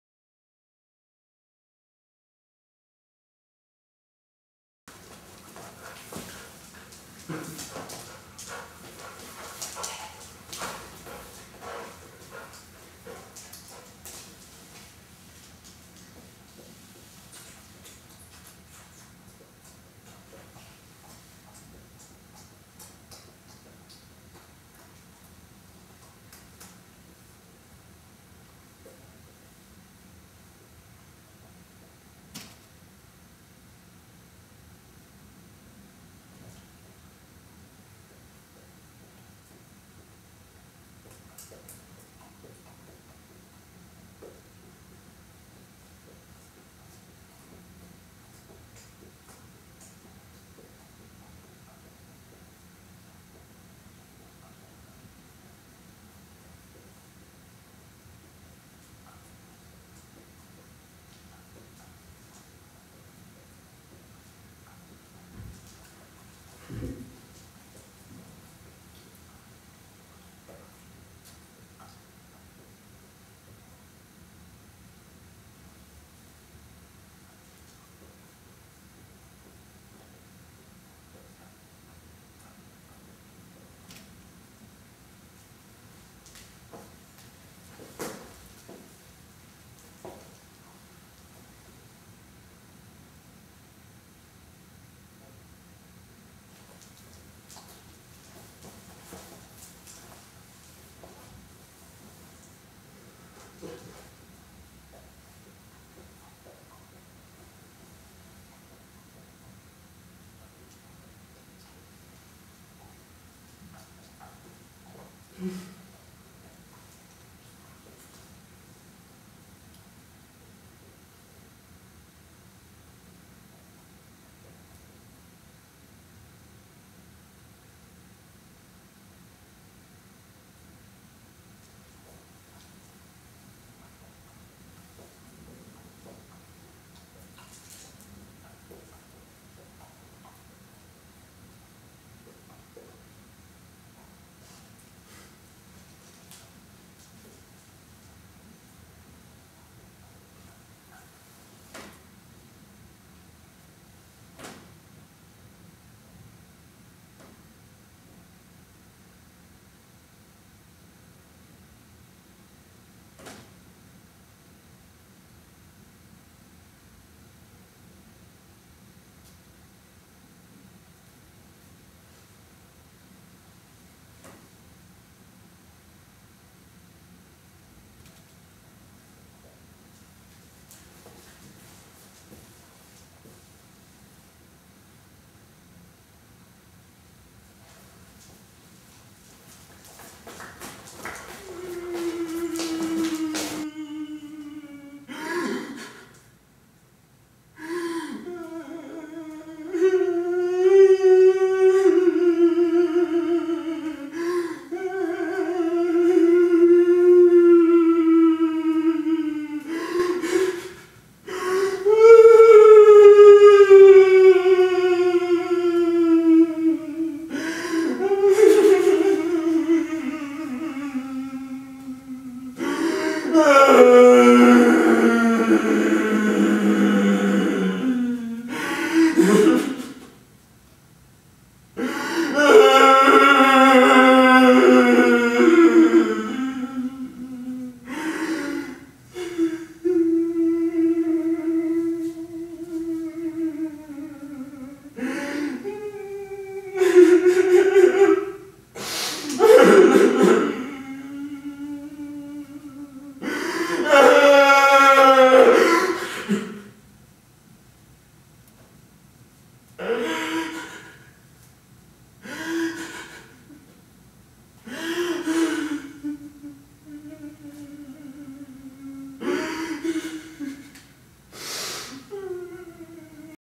Lecture on a box trailer
Trailer for the upcoming soloperformance by Iggy Malmborg

Opening at Bryggeriteatern / teaterhögskolan 29:th of January
Playing until 13:th of February